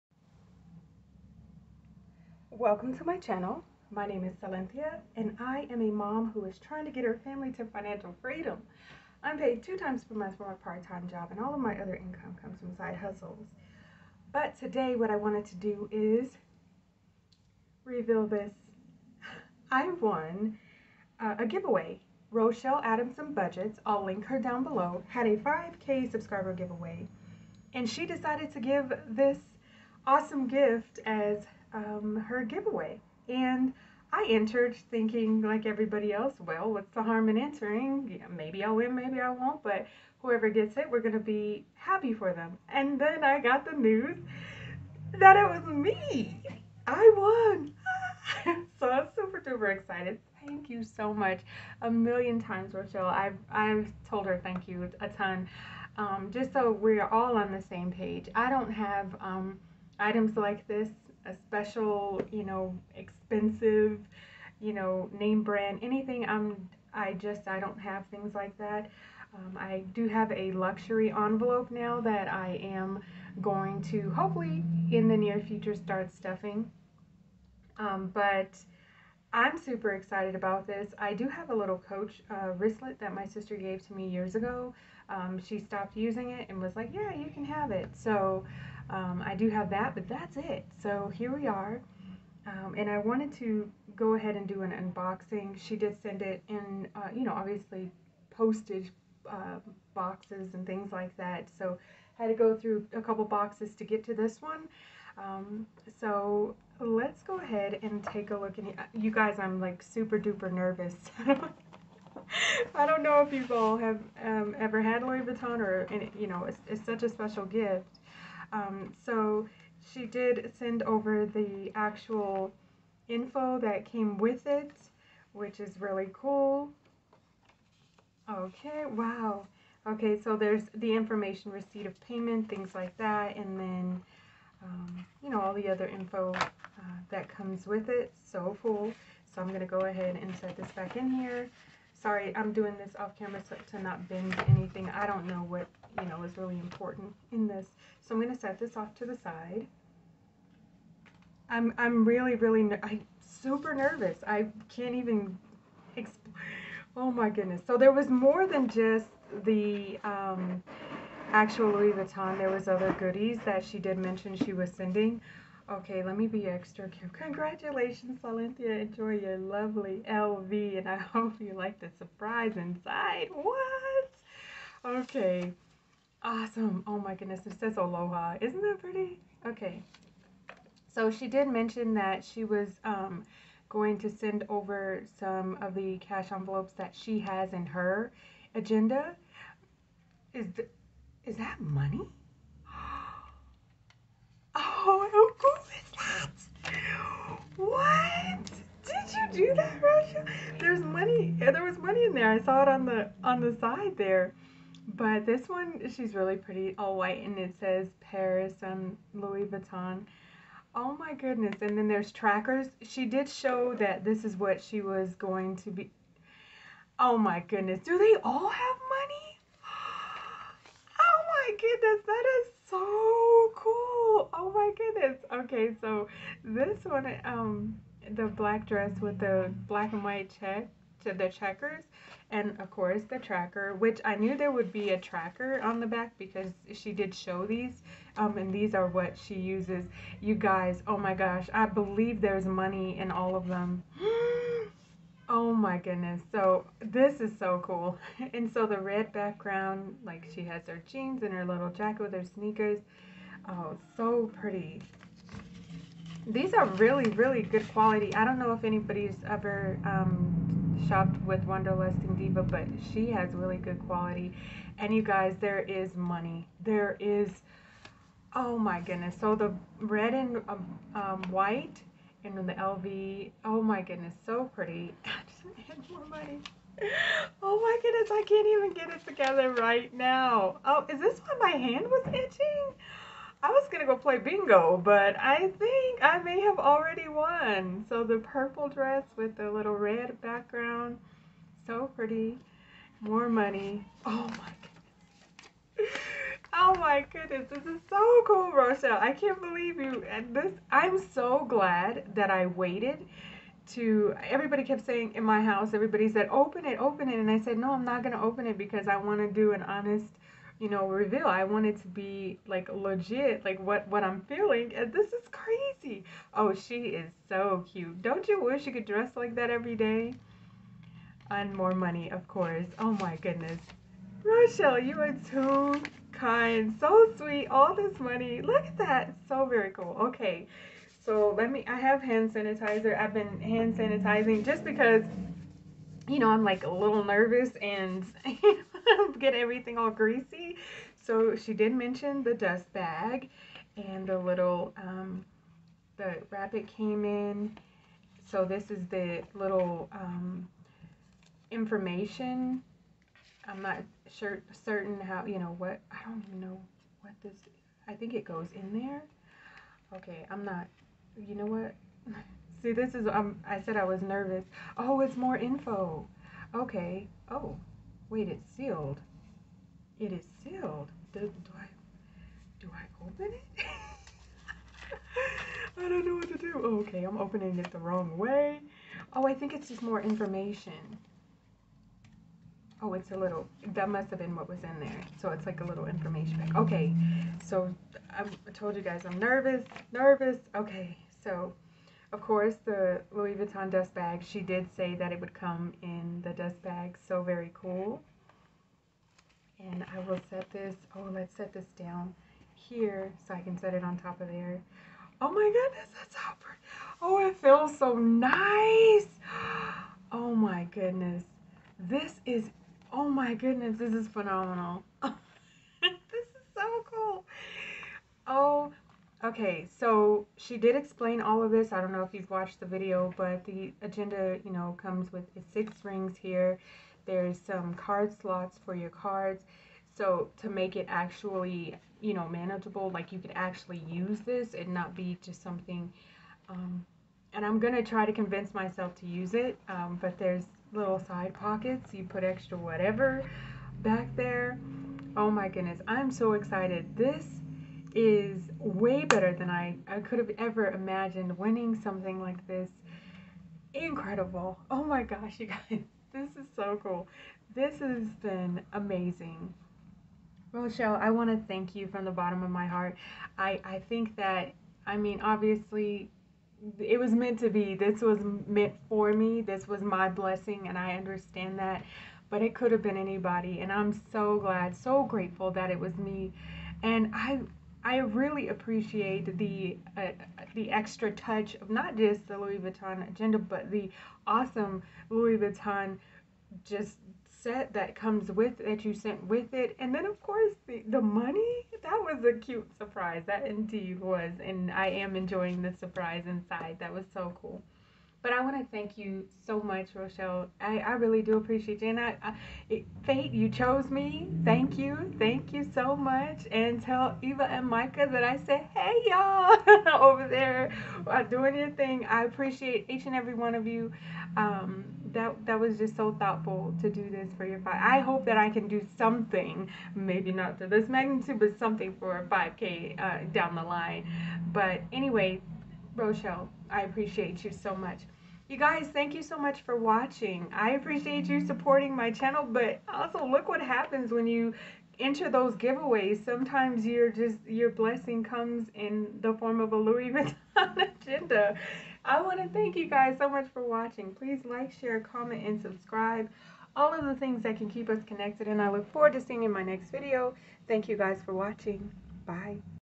Louis Vuitton Agenda || Rochelle Adamson Budgets 5K Subscriber Giveaway Winner || Super Happy Mail
I WON! Thank You Rochelle Adamson Budgets for my beautiful LV. I am over the moon! Check out what I won.😁😍💚 Happy Savings 🤑🤑